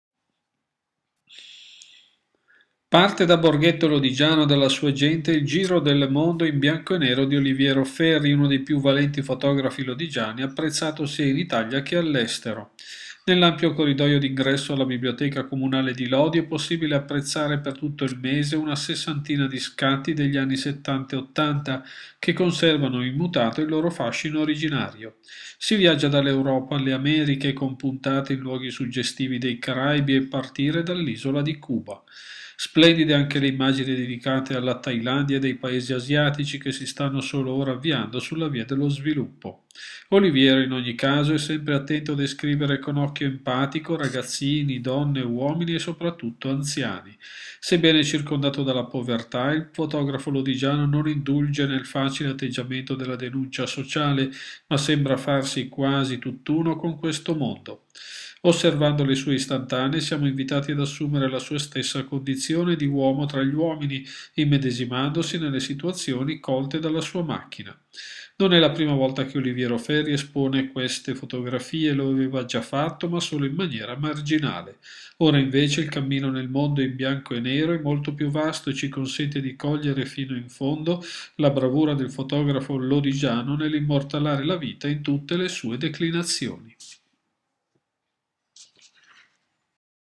Lodi, Oliviero Ferri in bianco e nero
Una sessantina di scatti nelle sale della biblioteca laudese